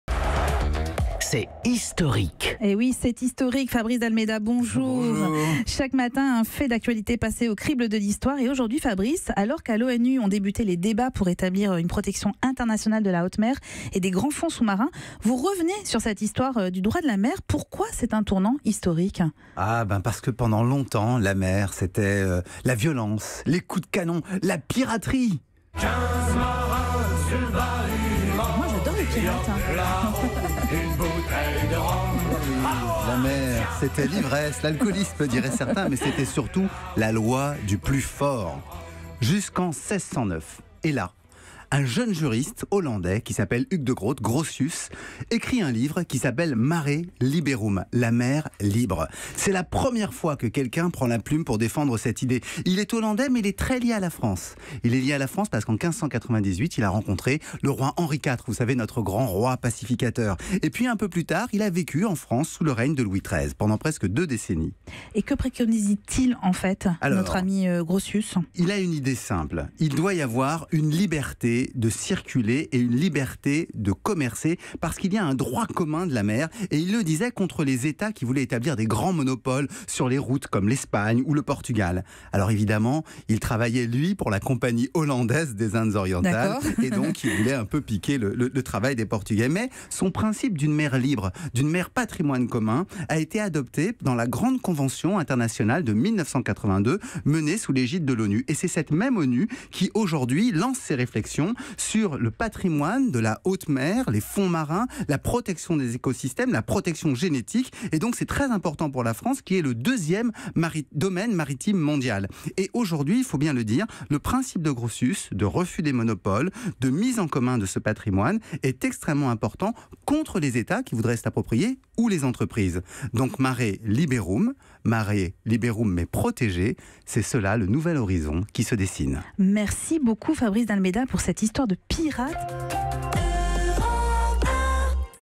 Les origines du droit maritime : discussions sur le statut de la Haute mer à l’ONU.
Tous les jours, Fabrice D'Almeida revient sur un fait historique marquant. Aujourd'hui, des discussions sur le statut de la Haute mer à l’ONU.